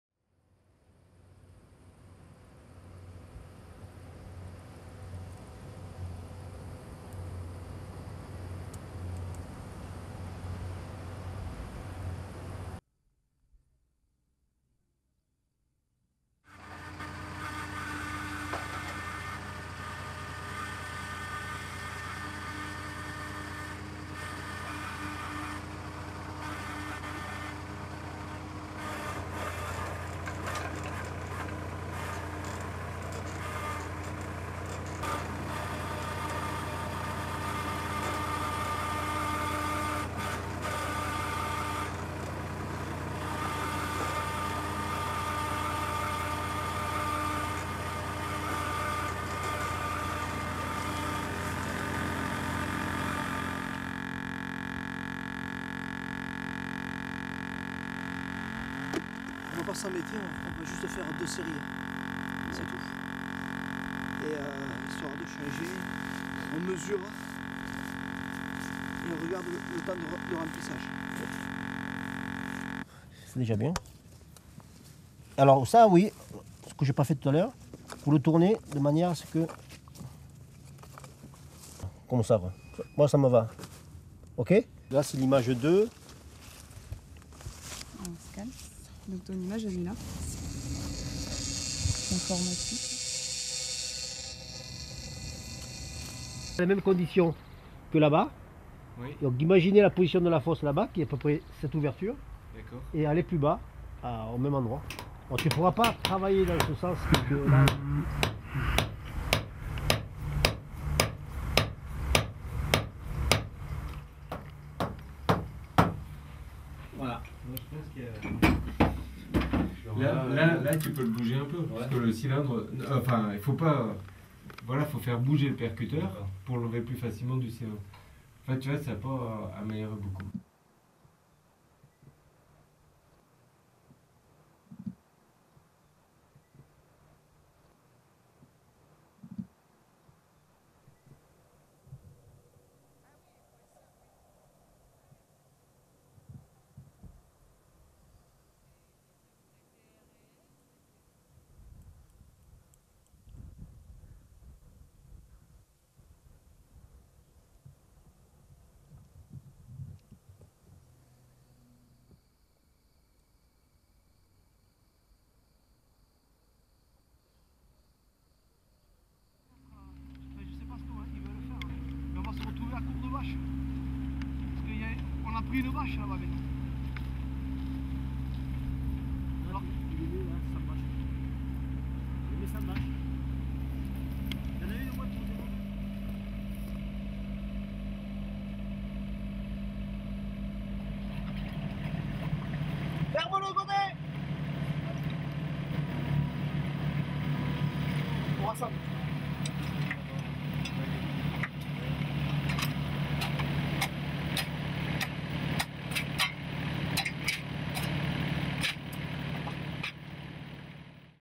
ECOSFIX - site expérimental RESTINCLIERES (France)
Quelques images et du son du site expérimental du projet ECOSFIX (étude des services écosystémique des racines) situé en FRANCE à Restinclières (34). Cette vidéo fait partie d'un WEBDOCUMENT conçu et réalisé par la scop AGROOF pour le projet ECOSFIX, actuellement en cours de réalisation.